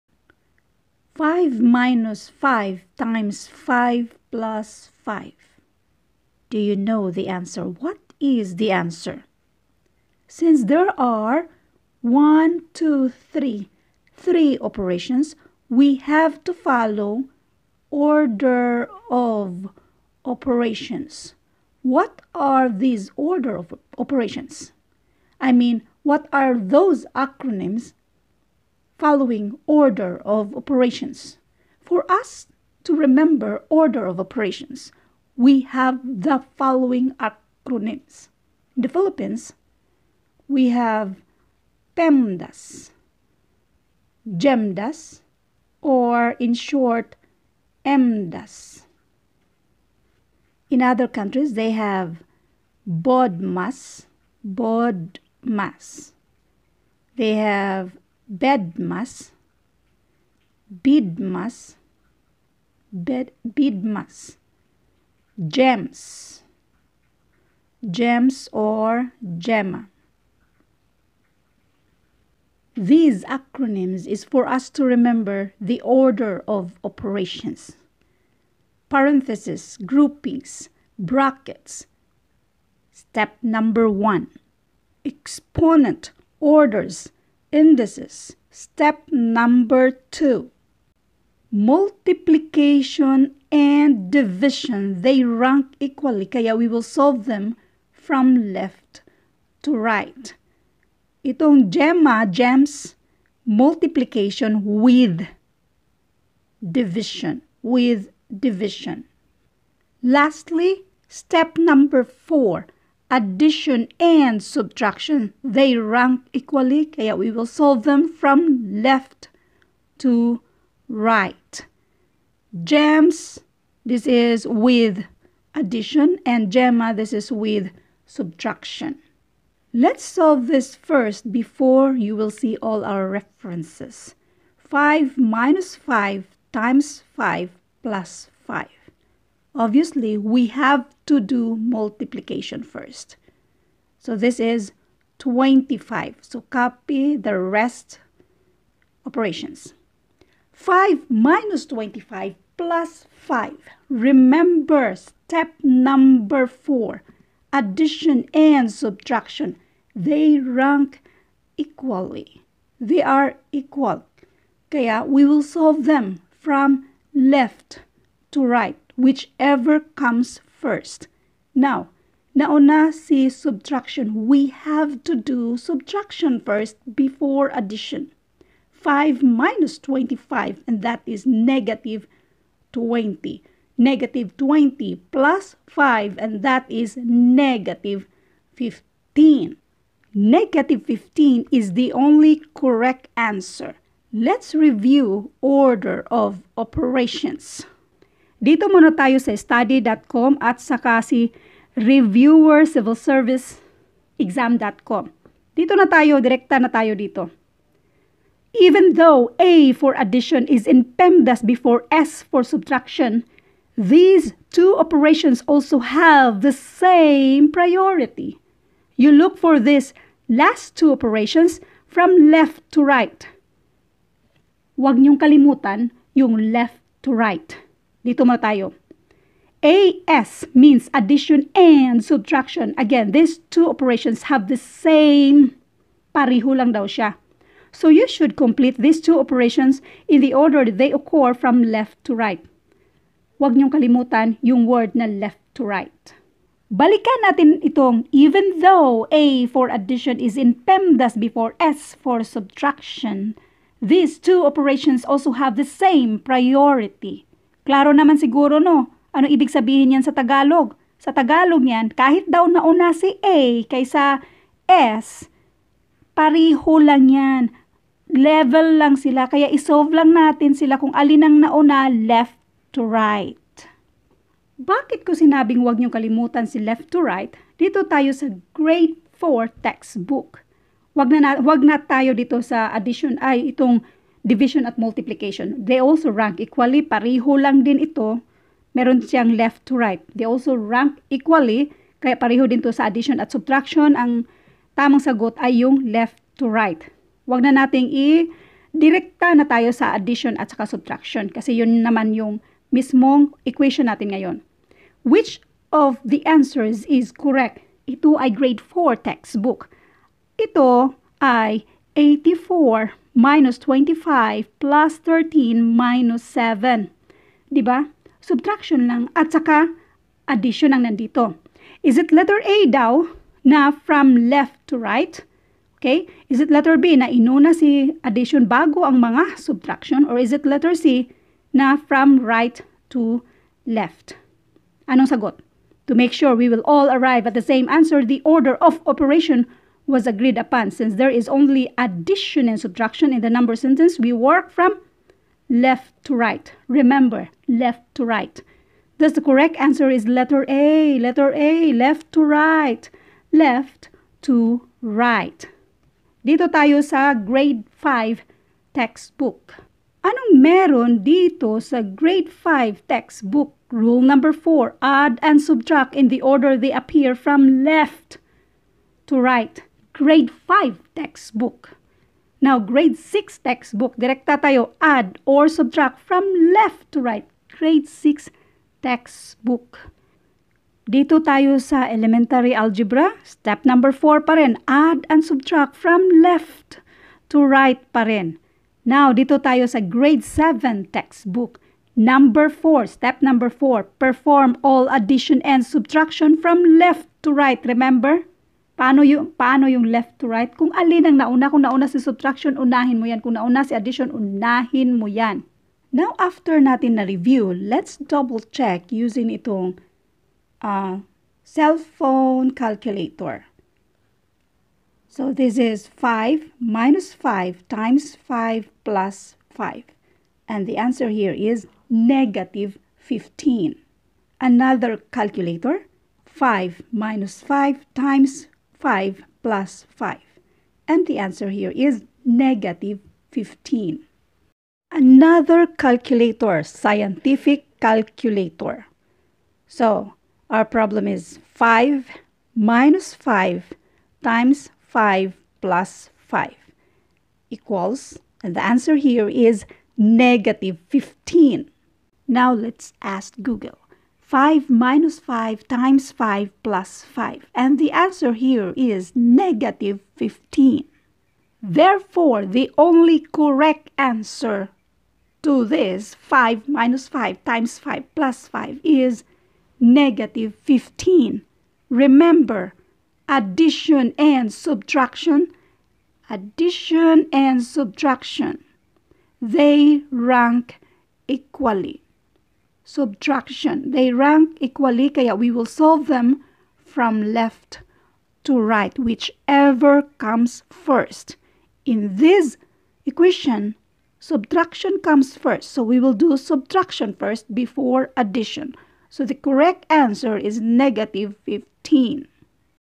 Viral Math: 5-5×5+5 =? Can you answer this correctly? | MDAS PEMDAS BODMAS Order of Operations
All about ORDER OF OPERATIONS (PEMDAS MDAS GEMDAS BODMAS)

Can you answer this correctly 5-5×5+5 = ? VIRAL MATH PEMDAS or Order of Operation?